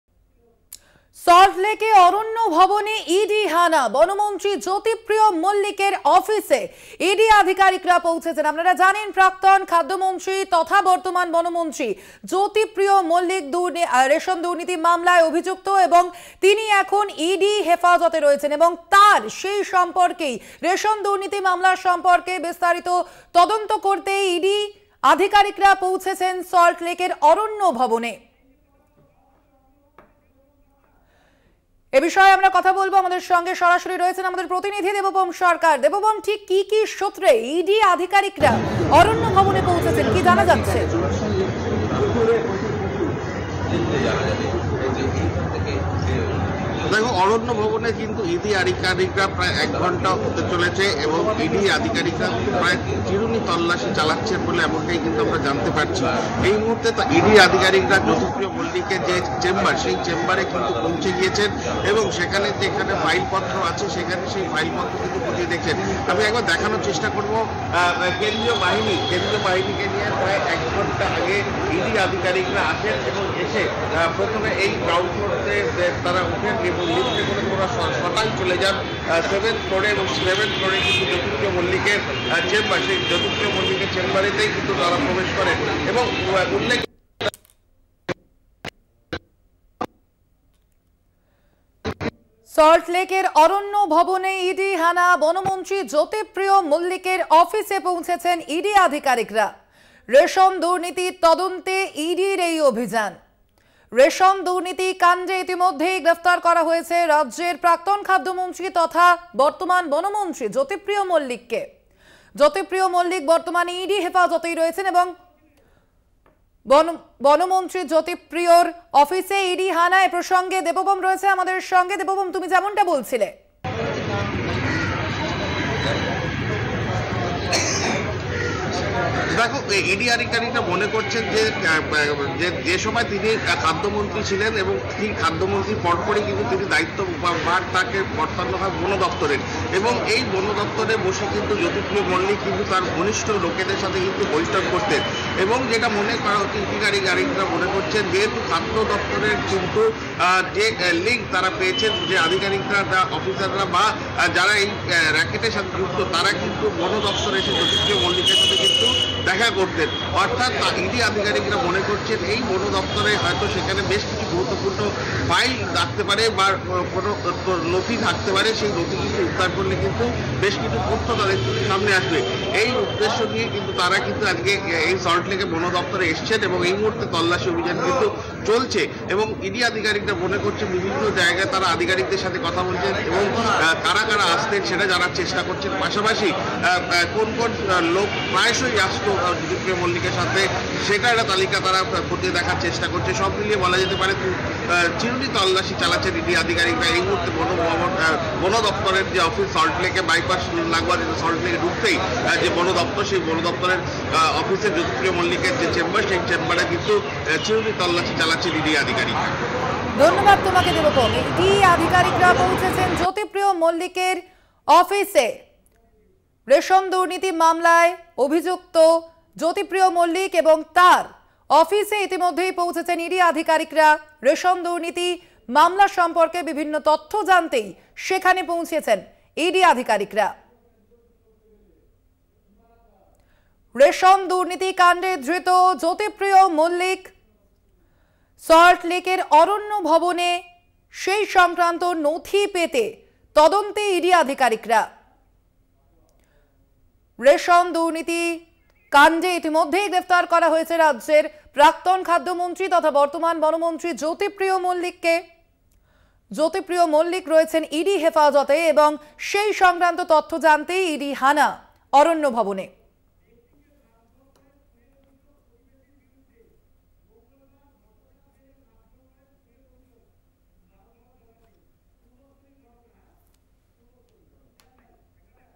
Jyotipriya Mallick | রেশন দুর্নীতি তদন্তে বালুর অফিসে ইডির অভিযান
website link for this copy :
@Kolkata Tv